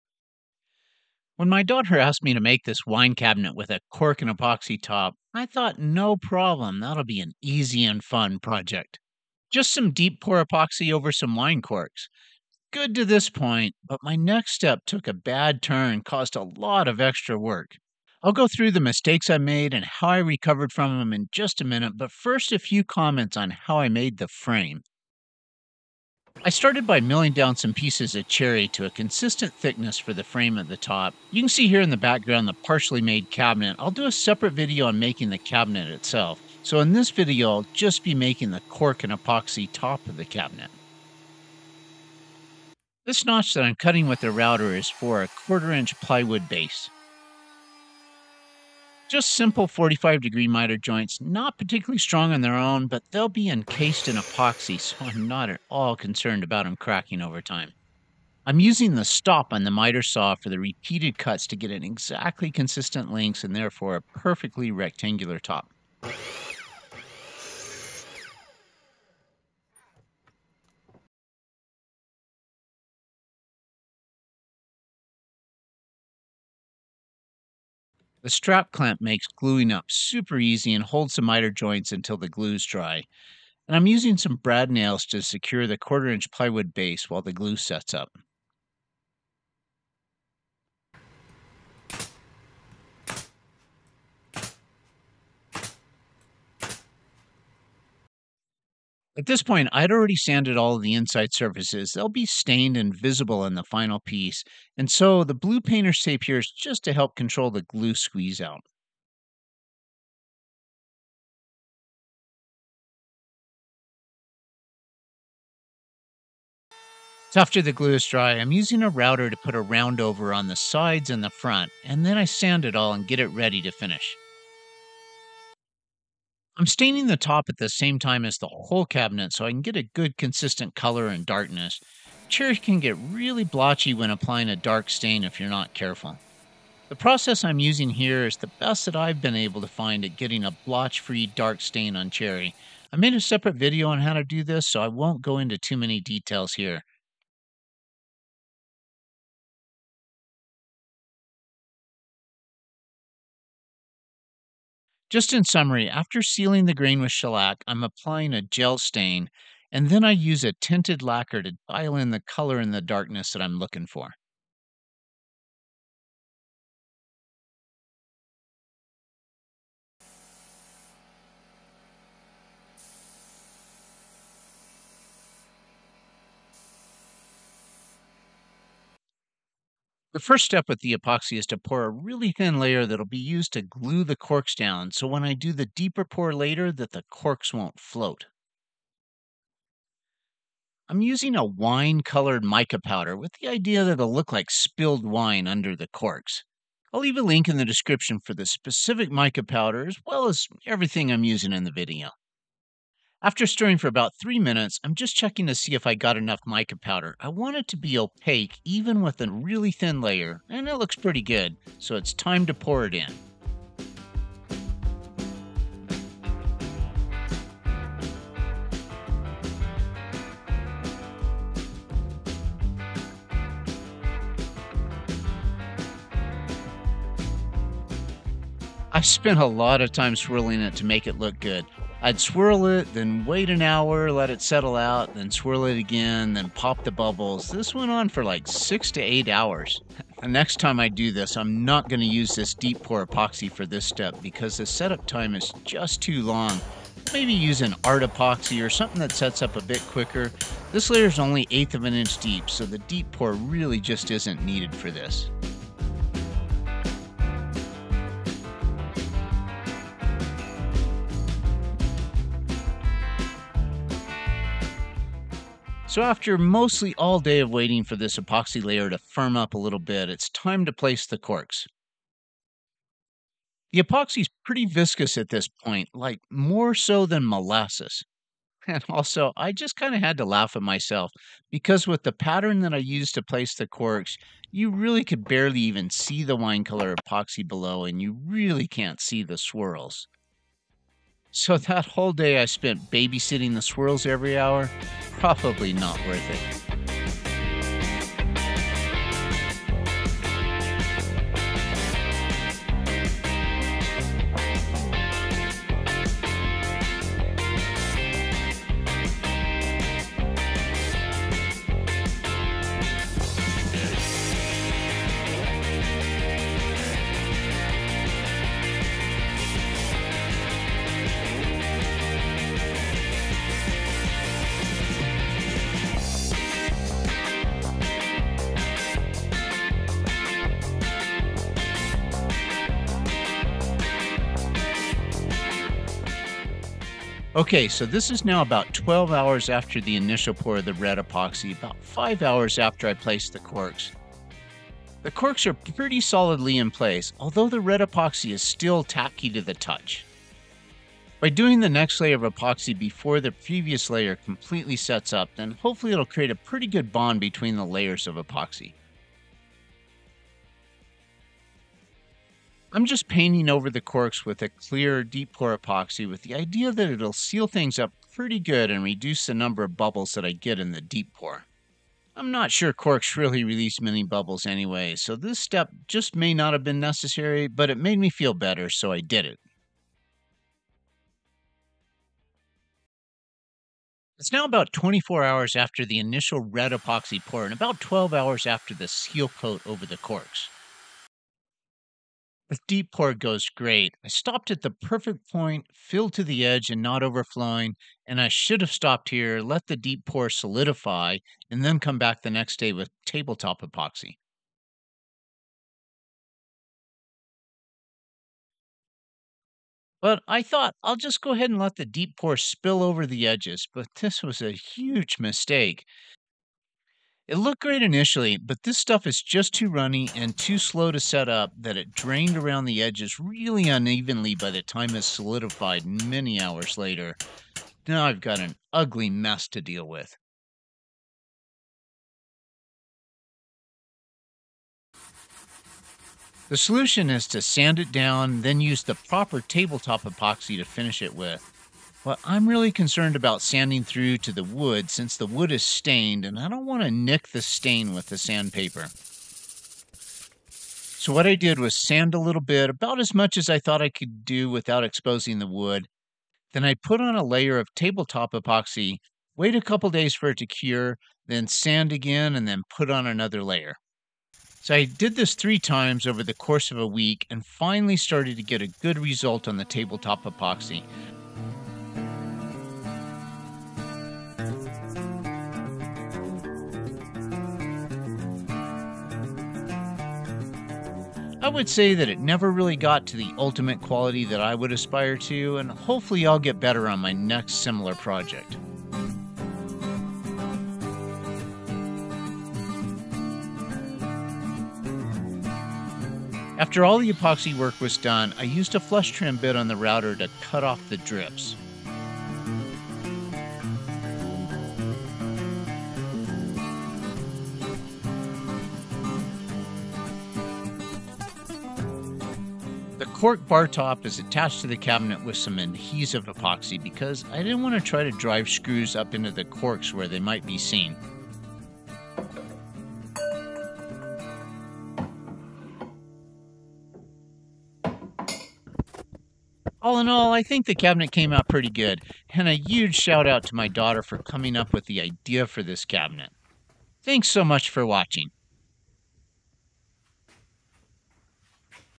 It was more difficult than I thought!
Making a wine cork and epoxy bar top / wine cabinet top.  In this video I go through the mistakes that I made and how I recovered from them.  I used a combination of deep pour epoxy, table top epoxy and a wine color mica powder.  The cabinet is made from Cherry.

Deep pour epoxy
Table top epoxy
Wine color mica powder
Gel Stain
Strap clamp